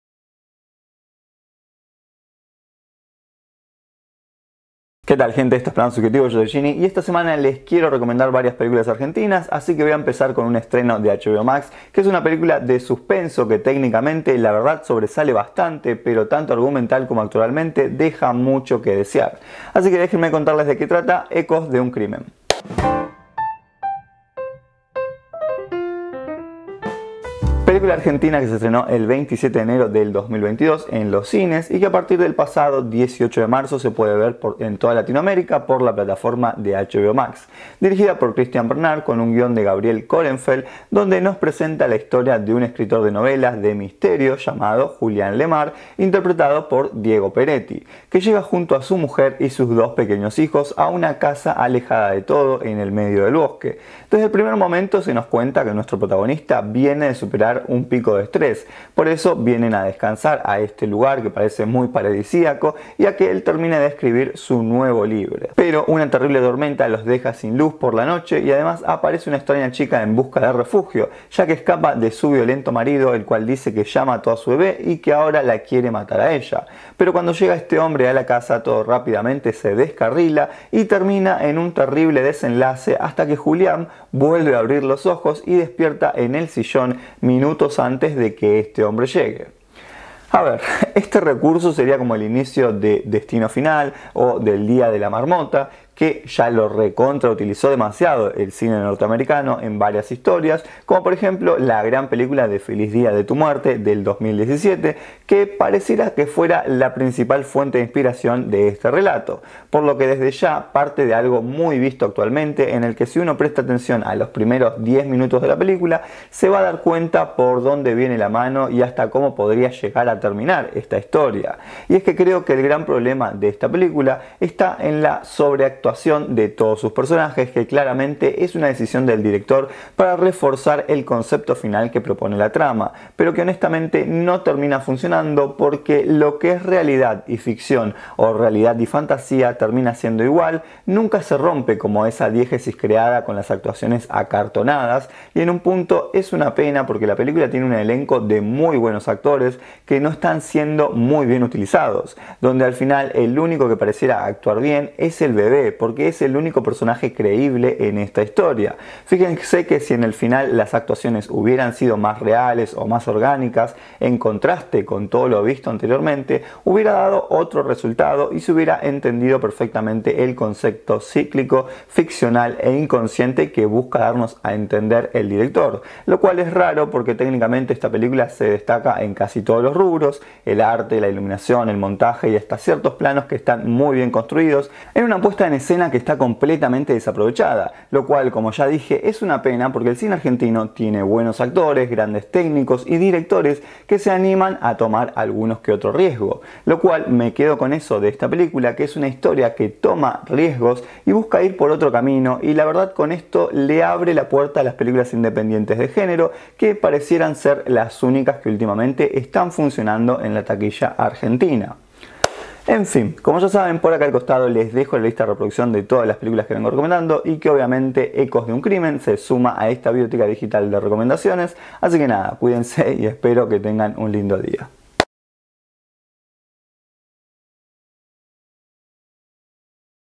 PELÍCULA | #134 Ecos de un Crimen | Reseña y opinión | MINI-PODCAST
Ecos de un Crimen (2022)
Dir. Cristian Bernard
Cast. Diego Peretti, Julieta Cardinali, Carla Quevedo, Diego Cremonesi, Carola Reyna

CATEGORÍA: ENTRETENIMIENTO LIGERO